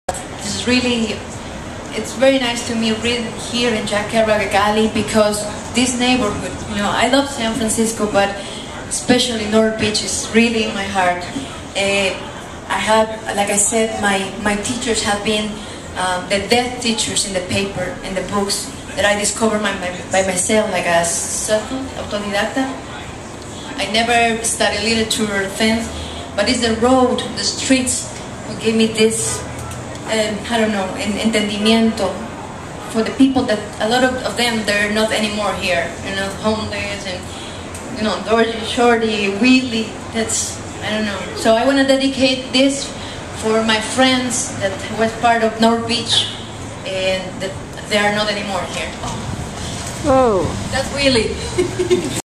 SFIPF Poetry Festival Carla Badilla Coronado Ecuador Poet
third biennial SF poetry festival north beach with Carla Badilla Coronado from Ecuador and Jessica Loos translator